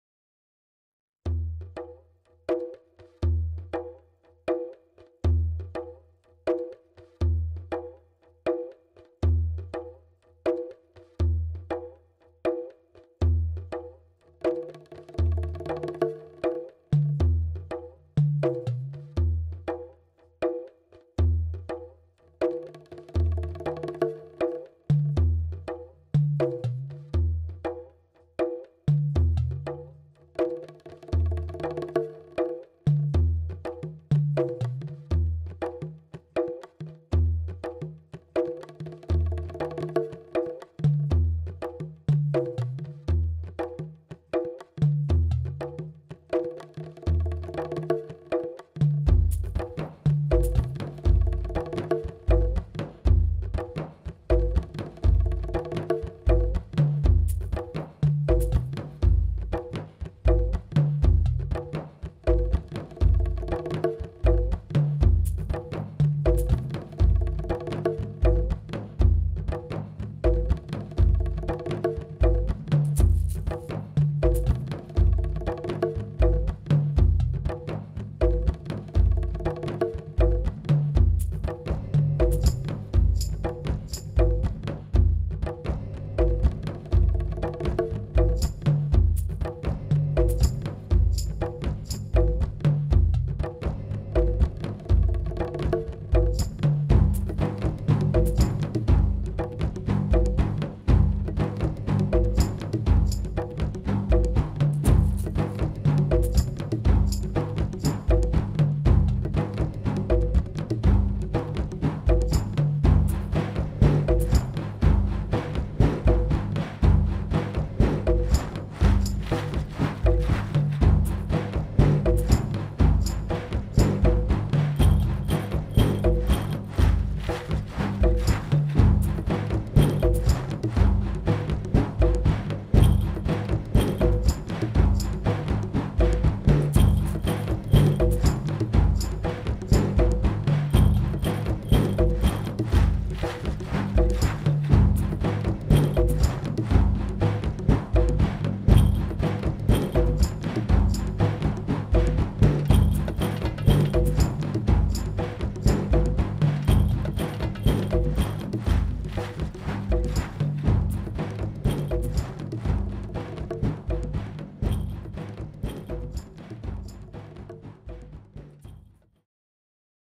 Acustica Audio | N4 Setups Test
Testing the N4 ability to create a setup of 1 or more programs which then are selectable as a separate plug-in for your DAW format. Audio recording features my percussion composition inspired by works of a great maestro Mr. Glen Velez.
Thanks to JPN Skins for lovely MFC skin.